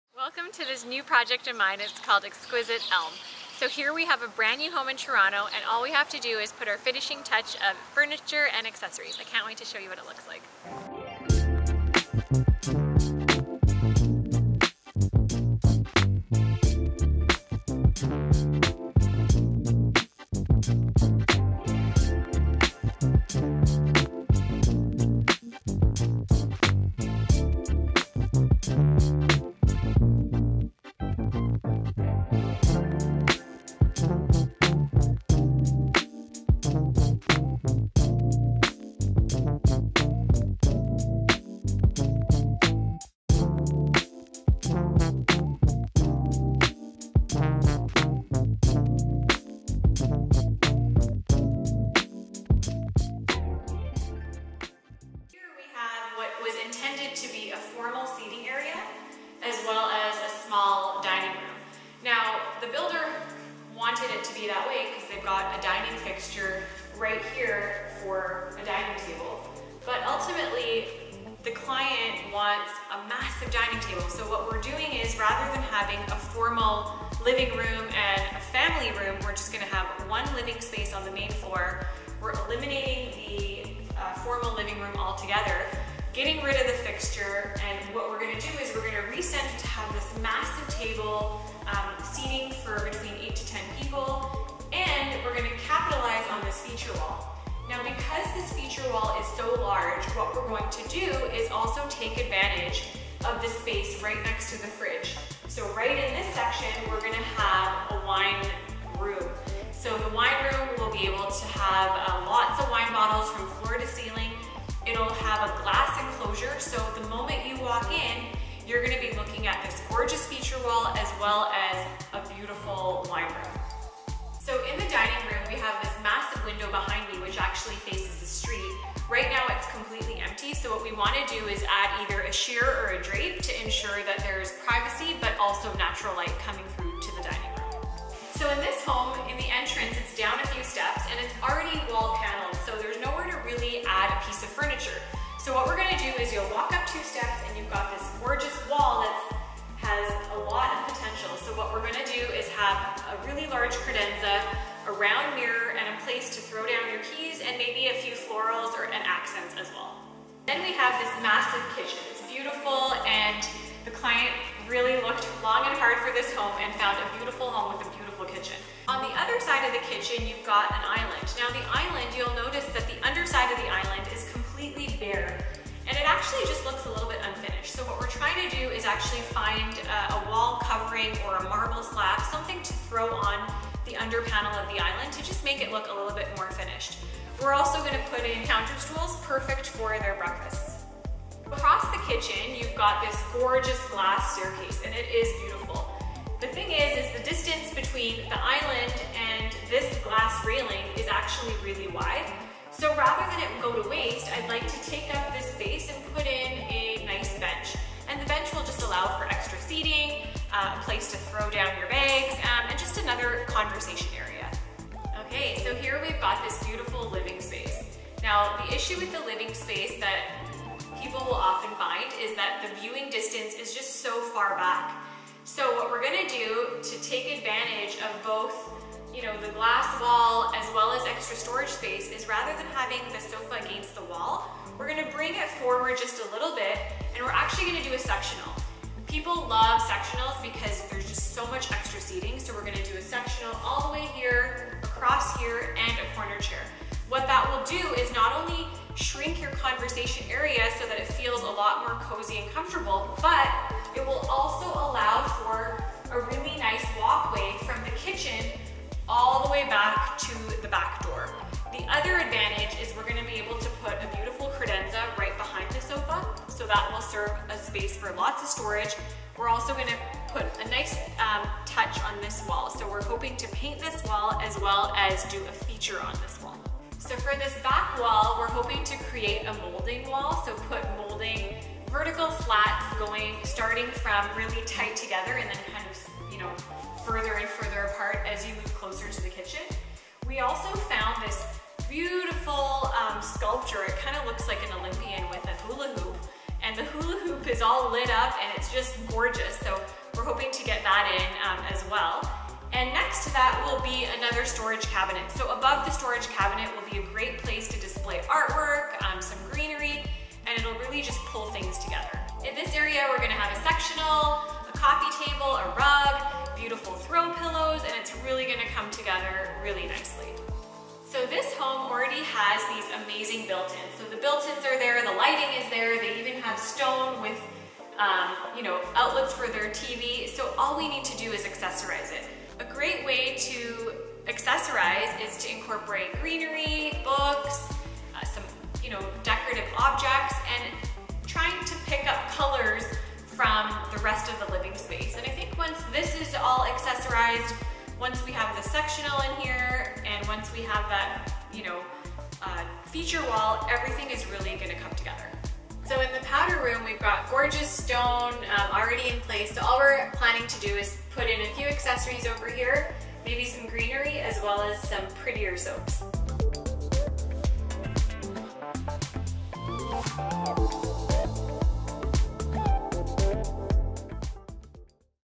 Stunning Living Room Redesign with Black Wall Moulding | Exquisite Elm
I've been dying to show off this furniture and decor project I've been working on the for the last 6 months in Bedford Park, Toronto. This project was done for a young couple who had purchased their first home which was beautifully renovated. They wanted decor to match their lovely finishing but also wanted it to be very functional for their needs.
They love to entertain so we got them a floor to ceiling wine wall and a massive custom marble dining table for them to welcome big groups. We also added this stunning bar cart from Sunpan. In the entrance, we found a place for their wallets, keys, etc. on this credenza from CB2 and added a giant circular mirror from them to check their hair / makeup on the way out the door.
Leading into the kitchen, we found this metallic showpiece bench from a supplier overseas. While at the bar counter we added stools and finished off the bottom of the counter with marble wallpaper. Small details like this really add an elegant touch to the space.
In the living room, we created a deep black feature wall with scaled vertical moulding lines that you won't find anywhere else and then put this stunning gold credenza from CB2 with a custom painting on top to lend some drama to the feature wall. Finally, we found this sculpture of a man holding a ring light to really put a mark on this area.
In the seating area, we found them a large sectional from Rove Concepts enough to see large groups of people. We added a side chair and beautiful coffee table that has some great detailing. On the TV wall, we finished everything off with accessories we found from all over with many being personally important to the couple.
Finally, the powder room was mostly done but we added wallpaper and some accessories to simply finish off the space and make it more liveable and interesting.
TIMESTAMPS:
0:00 - Introduction
0:10 - Preview
0:55 - Dining Room
2:18 - Entry Credenza
2:40 - Kitchen
3:11 - Bench
3:35 - Living Room
5:21 - TV Wall
5:55 - Powder Room
CREDITS:
Hosts: Iman Lalji
Editors: Happy Soul Films
Produced By: Happy Soul Films